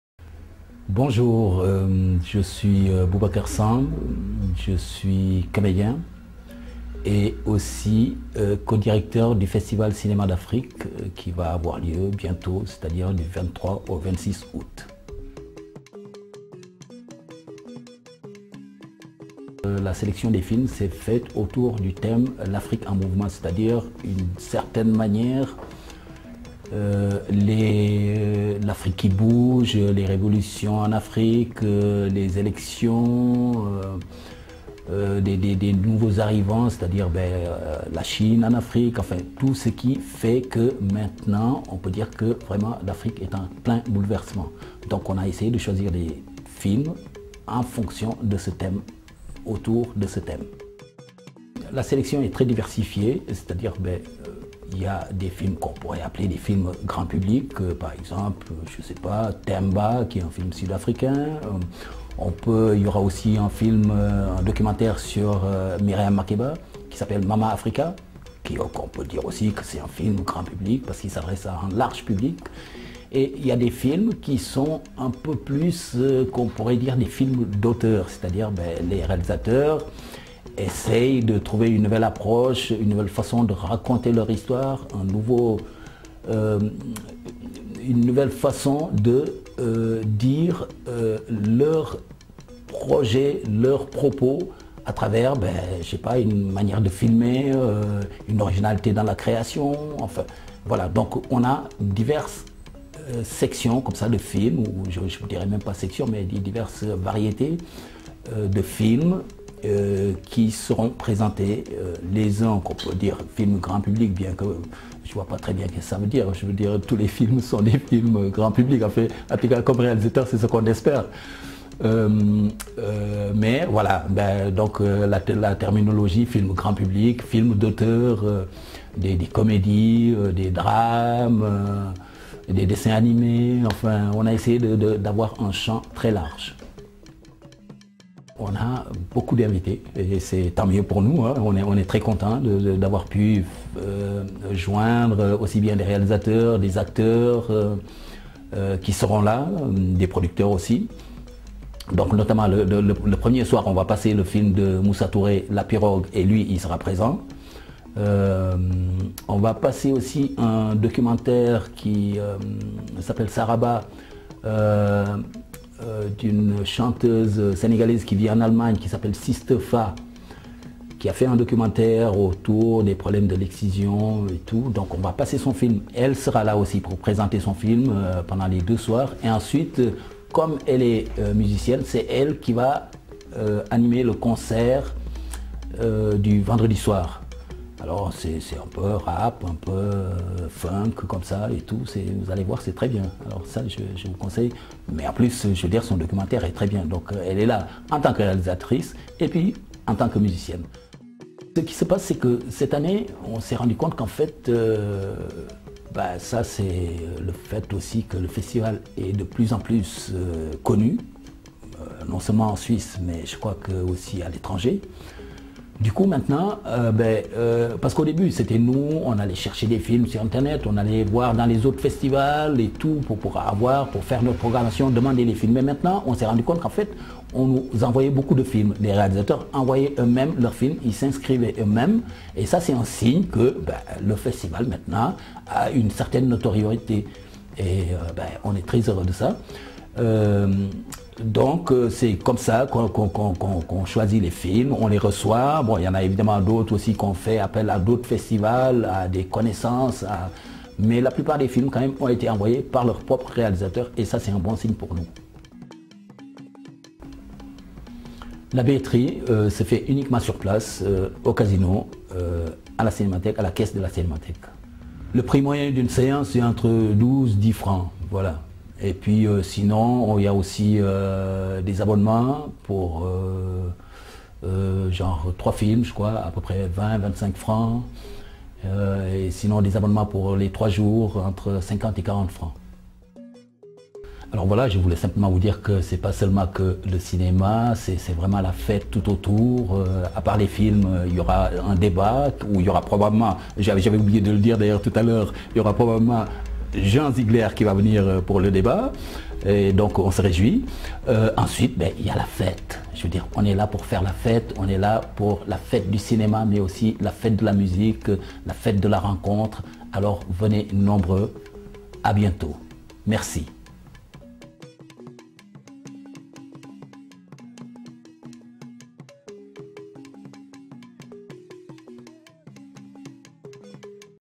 Interview Boubacar Samb
Boubacar Samb présente le 7e Festival cinémas d'Afrique - Lausanne du 23 au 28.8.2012 sur MIA - Culture